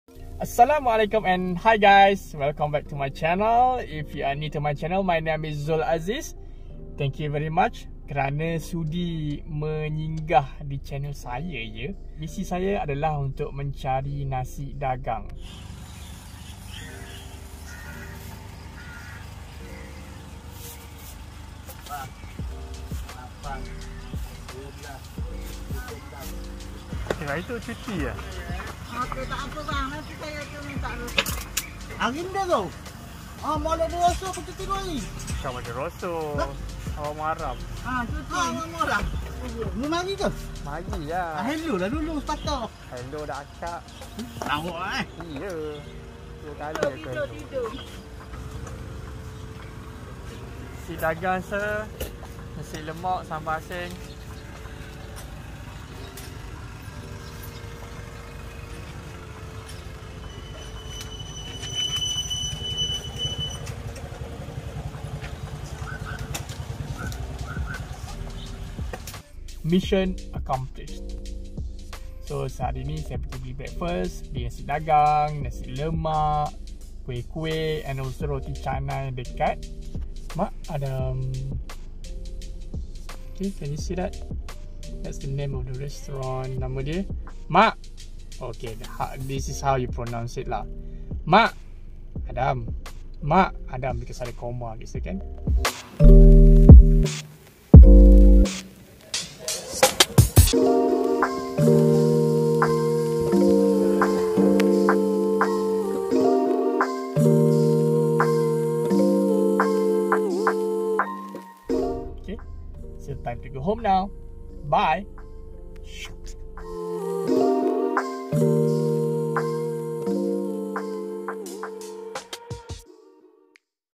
vlog #36 - NASI DAGANG selepas tol!!!
Last week, gerai Nasi Dagang Suhaimi tutup, cuti sebab Awal Muharram. So, hari ni, sekali lagi saya cuba "terjah" ke gerai beliau.

Sekiranya anda ingin mencari nasi dagang Terengganu yang original dan sedap, taip di ruangan komen. Saya akan share location gerai tersebut.